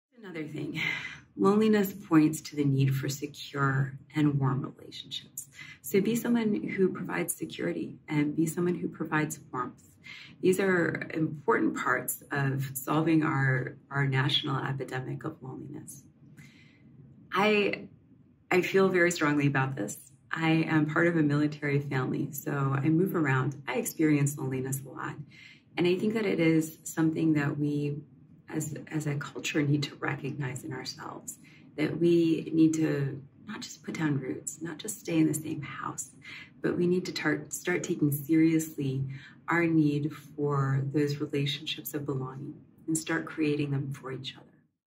A Faithful Response to Loneliness | Susan Mettes
How can Christians respond to the problem of loneliness, especially in light of our current cultural moment? Susan Mettes, author of The Loneliness Epidemic, gives us insight into how we can better discuss and combat loneliness.

Watch this full talk, The Loneliness Epidemic, on Q Media.